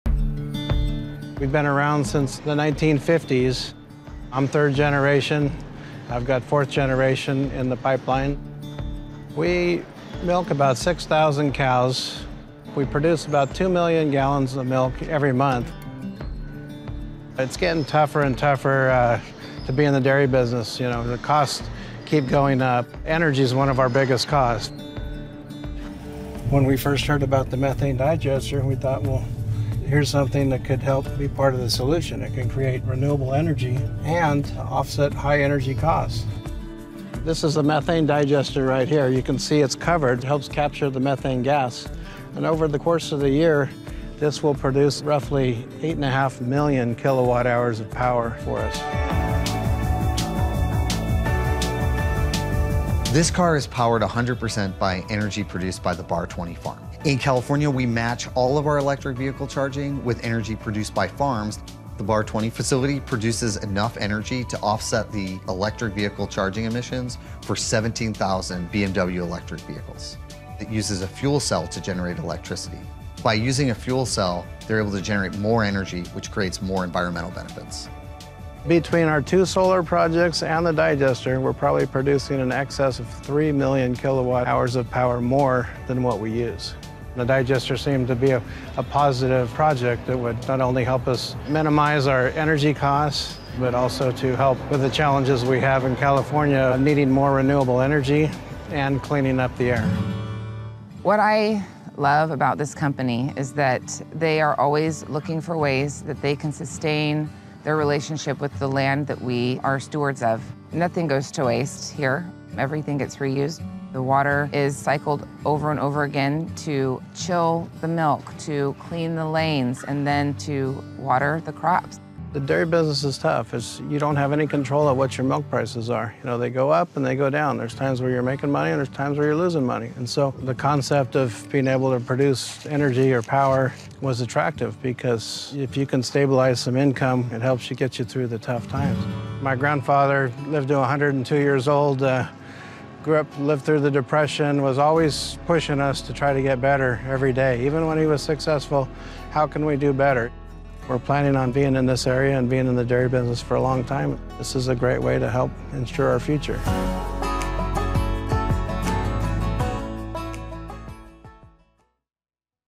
Bar 20 Dairy Reduces Energy Costs, Limits Emissions, and Invents a New Business
In a first-of-its-kind innovation, Bar 20 Dairy collaborated with California Bioenergy and Bloom Energy to turn methane from cows into clean energy without combustion that is stored in fuel cells, with enough renewable energy to power the farm and charge 17,000 BMW electric vehicles.